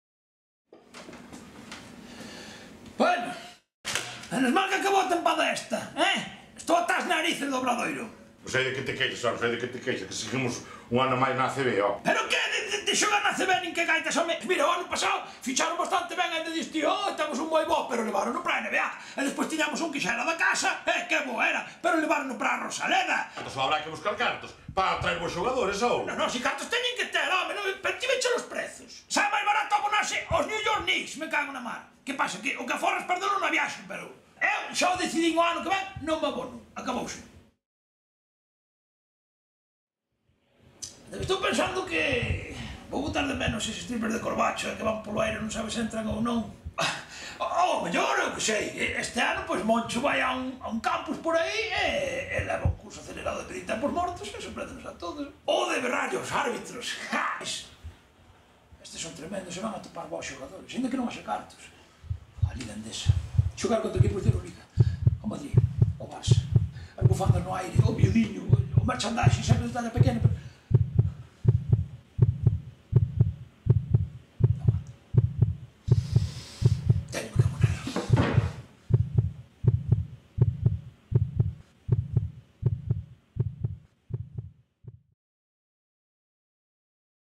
Pídemo o corazón - Spot Rio Natura Monbus HD -
Campaña de abonados 2014/2015 "Pídemo o corazón", do Rio Natura Monbus, protagonizada polo actor Pepo Suevos coa colaboración de Suso Coba.

Agradecimientos: Casa de Xantar O Dezaseis